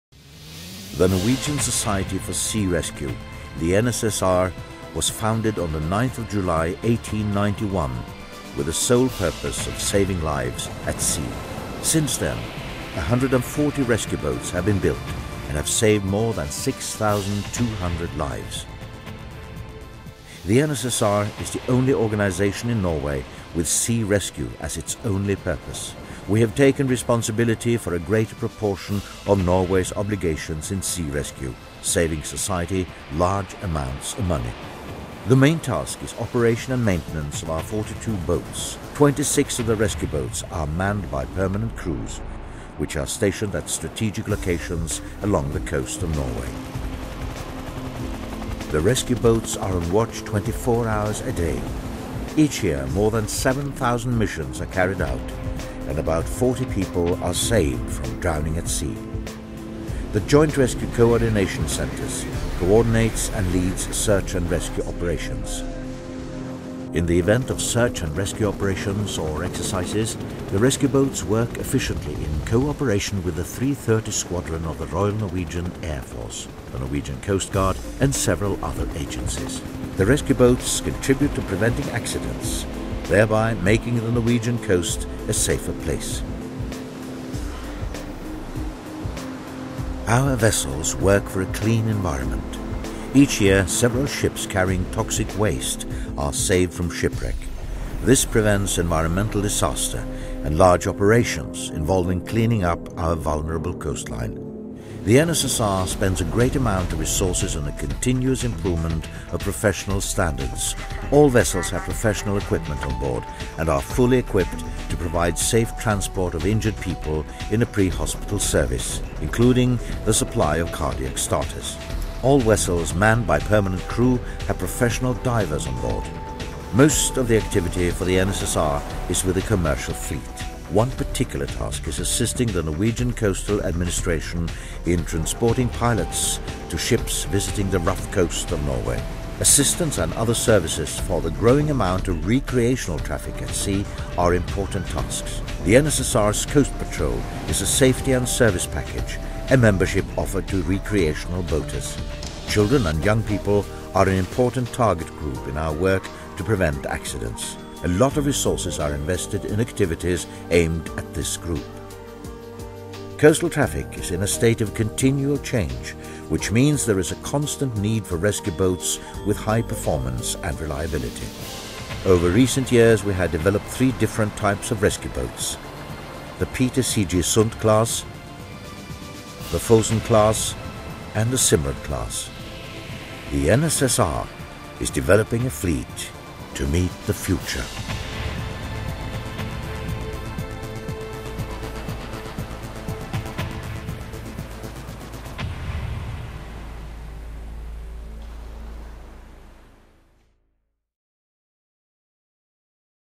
Norwegian Society for Sea Rescue 2008
The Norwegian Society for Sea Rescue's presentational video for 2008.